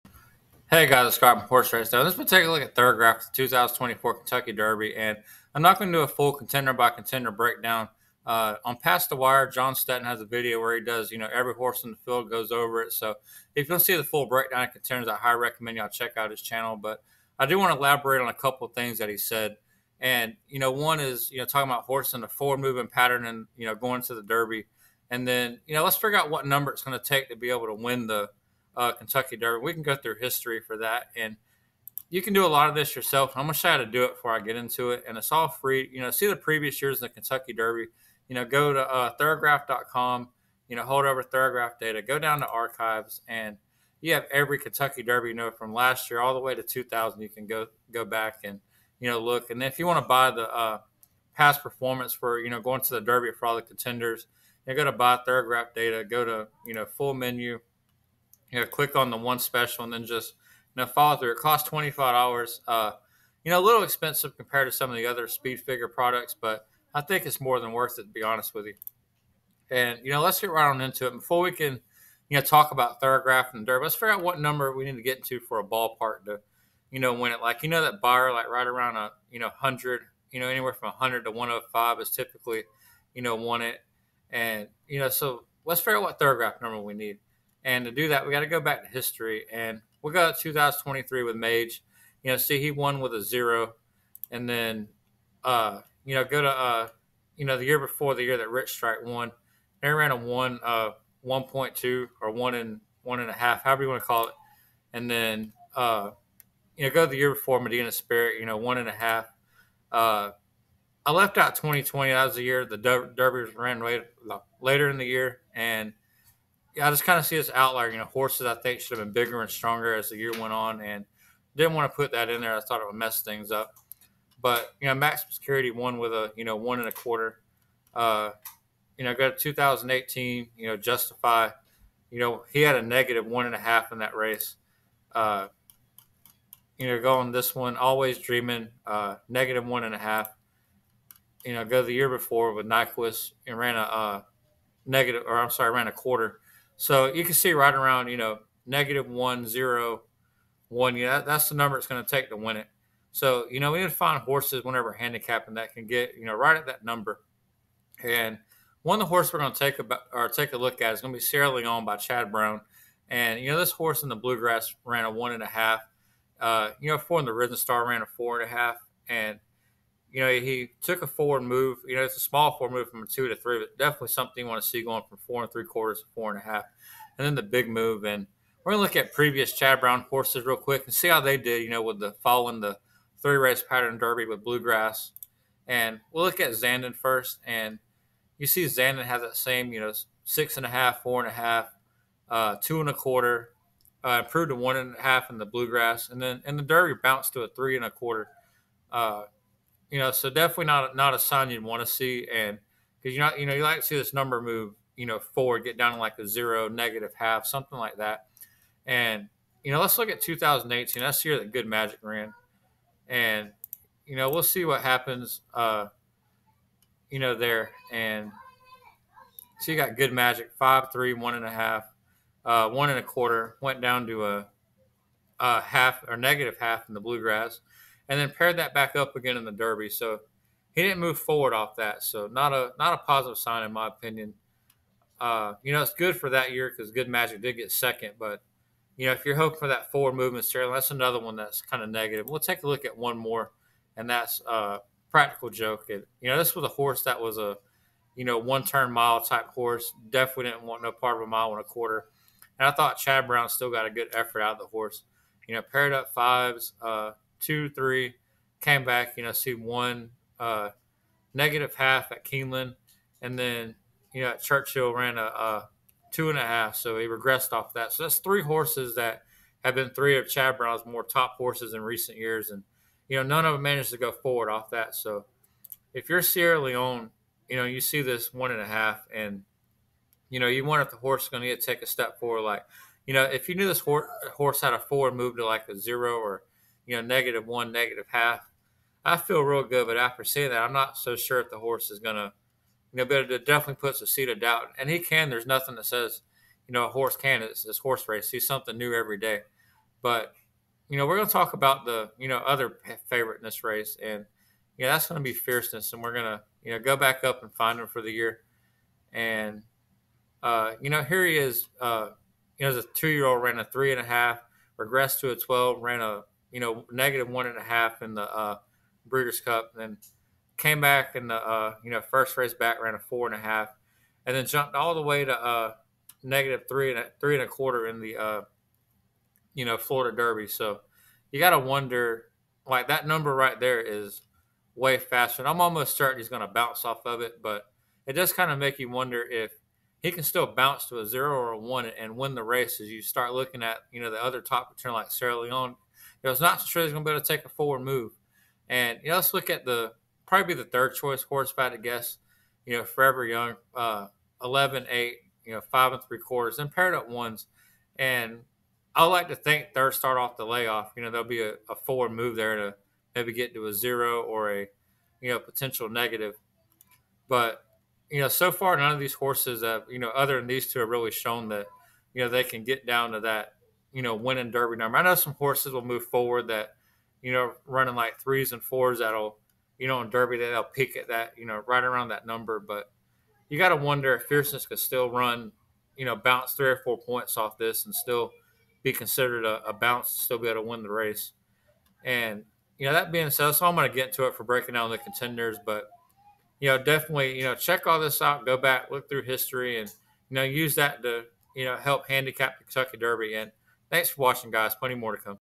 2024 Kentucky Derby Preview. Using Thorograph patterns to handicap the winner
I breakdown the 2024 kentucky derby using thorograph